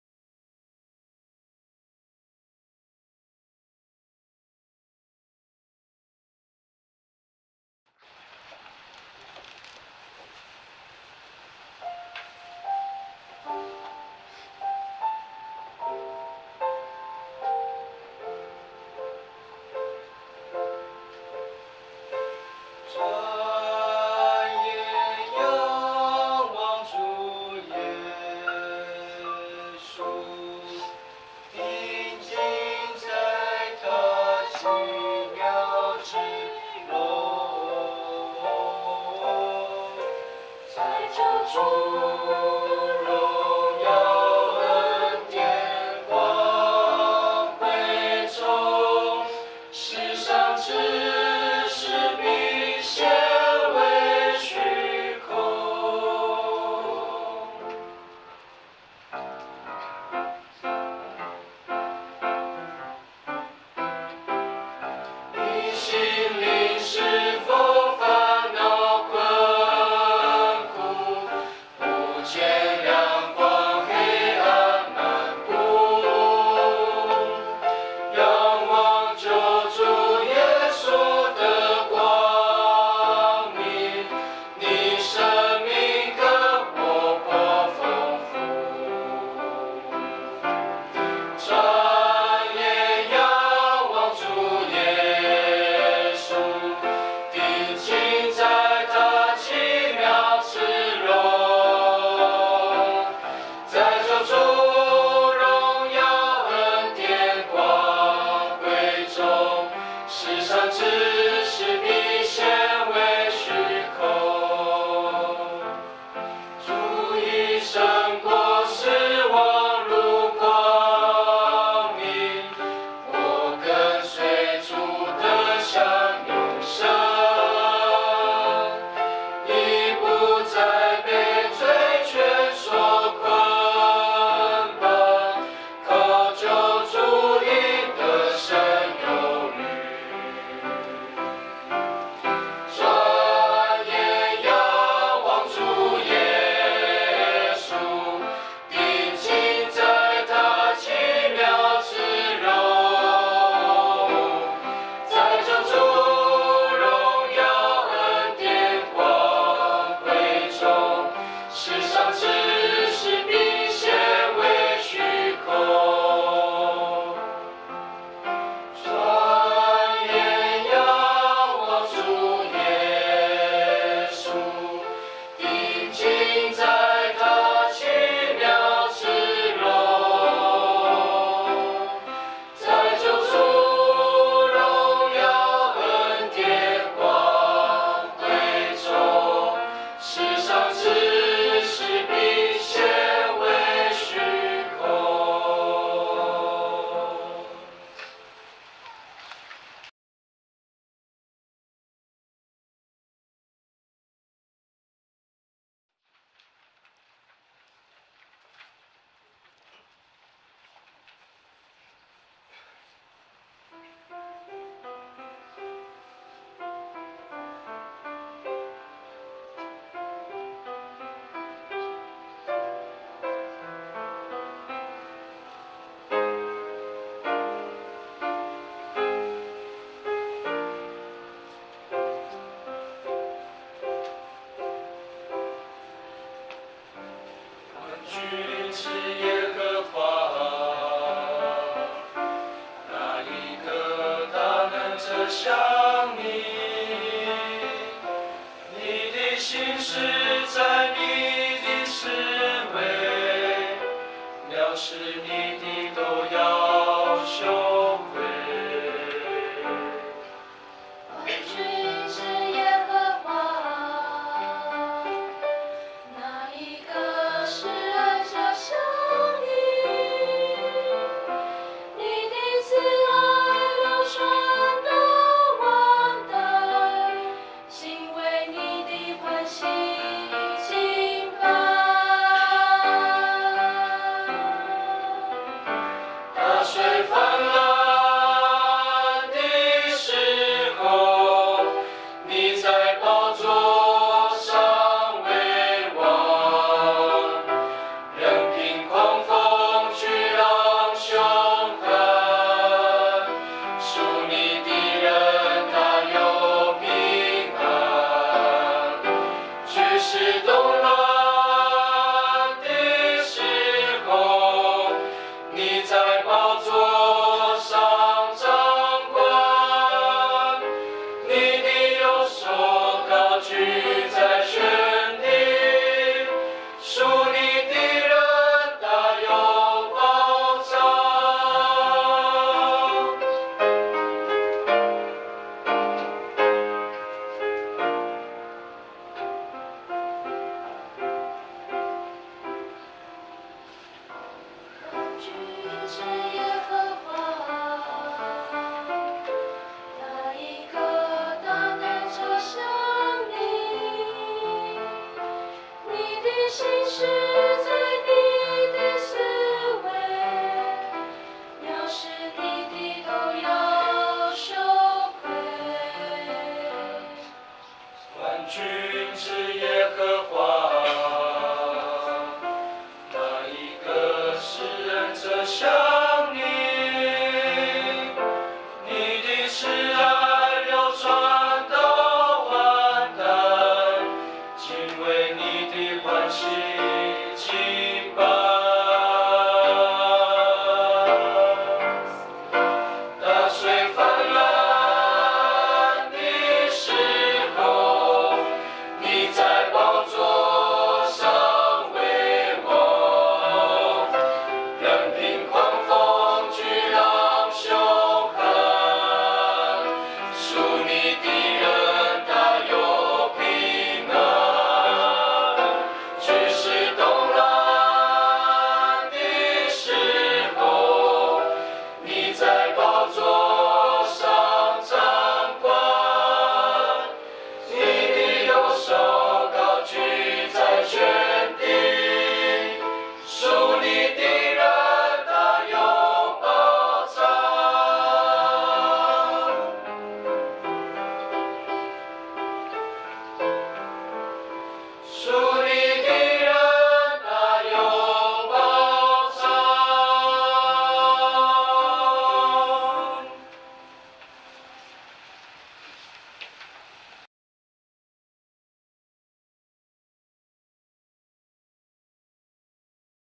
20170312 崎頂教會青年詩班於聖餐禮後的感恩獻詩記事
時間:2017年03月12日
地點:真耶穌教會崎頂教會
事由:聖餐禮